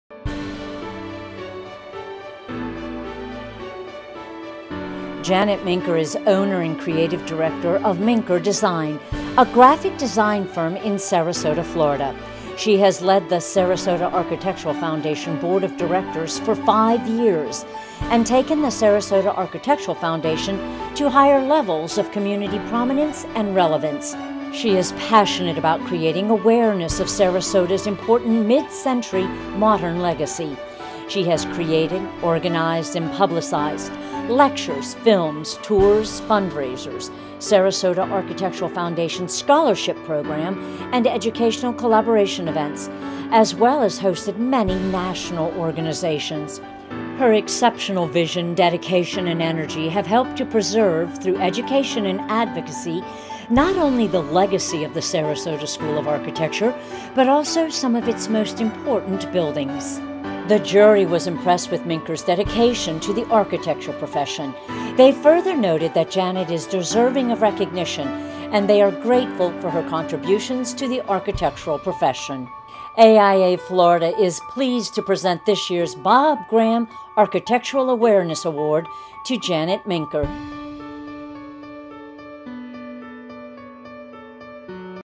AIA Florida Bob Graham Architectural Awareness Award - Janet R Minker
Bob Graham Architectural Awareness Award
Janet R. Minker, SAF Chair

Janet Minker is owner and creative director of Minker Design, a graphic design firm in Sarasota, Florida. She has lead the Sarasota Architectural Foundation board of directors for five years and taken the Sarasota Architectural Foundation to higher levels of community prominence and relevance. She is passionate about creating awareness of Sarasota’s important midcentury modern legacy. She has created, organized and publicized lectures, films, tours, fundraisers, Sarasota Architectural Foundation’s scholarship program and educational collaboration events, as well as hosted many national organizations. Her exceptional vision, dedication and energy have helped to preserve through education and advocacy, not only the legacy of the Sarasota School of Architecture but also some of its most important buildings.

The jury was impressed with Minker’s dedication to the architecture profession. They further noted that Janet is deserving of recognition and they are grateful for her contributions to the architectural profession.